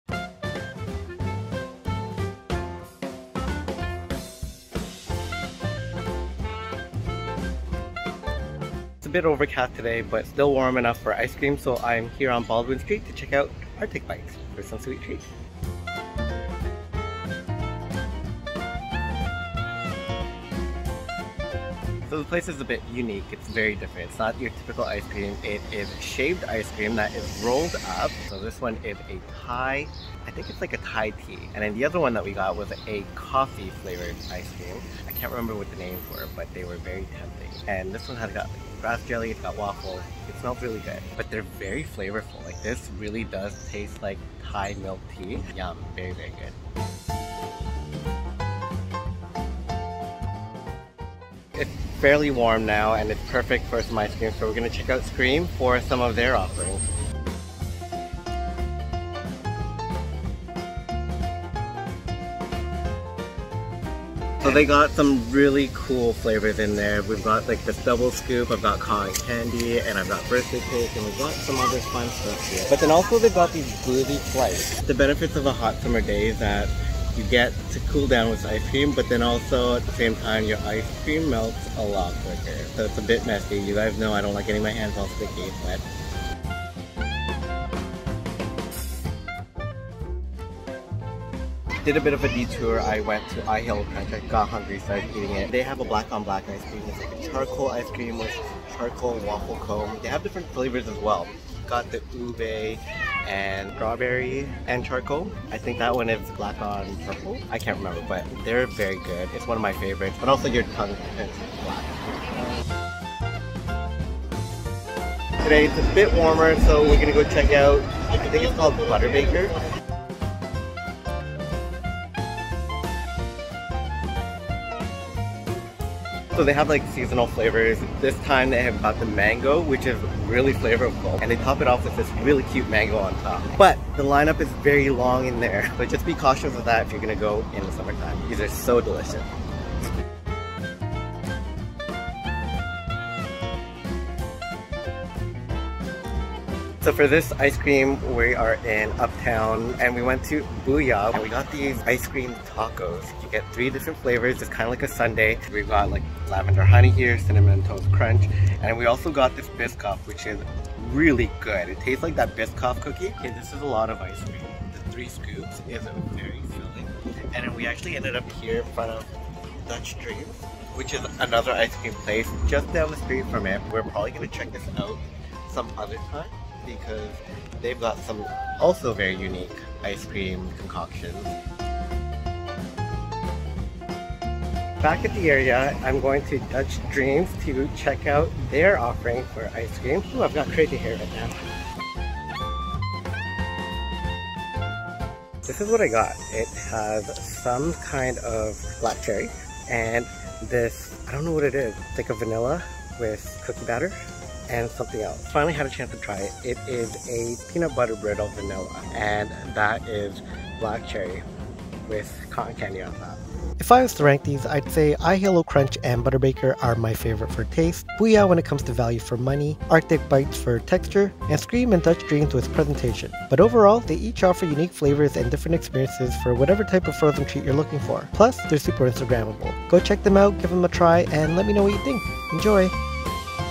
Best Ice Cream Spots in Toronto
Sweet treats and cool eats! These are some of my favourite ice cream spots in Toronto [subtitles available]

These are just some of the few ice cream shops I've been enjoying here in Toronto. Some are old favourites, and some are new discoveries. One thing for sure, they're all very instagrammable.

Here is the list of my favourite ice cream spots in Toronto:
iHalo Krunch - multiple locations
Butter Baker - 120 Dundas St W
Booyah - 16 Vaughan Rd A
S'Cream - 126 Cumberland St
Dutch Dreams - 26 Vaughan Rd